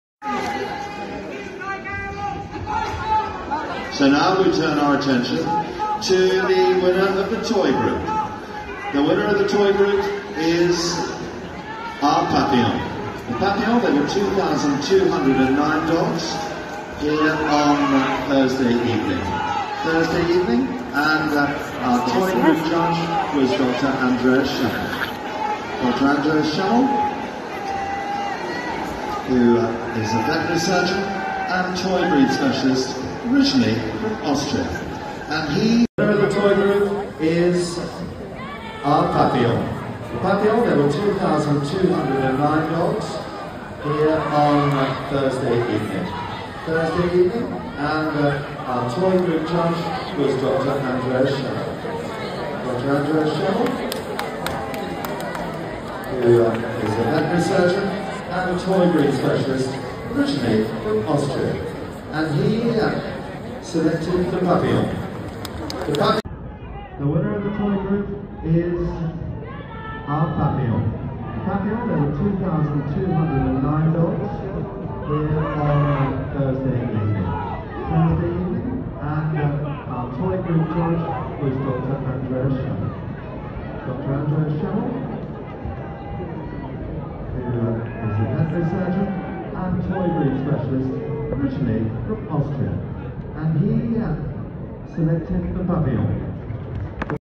Crufts 2024: Disruption at Crufts Best in Show. It’s kicking off!
Crufts in Birmingham UK yesterday,  family friendly event and is the worlds largest Dog show,
It’s kicking off!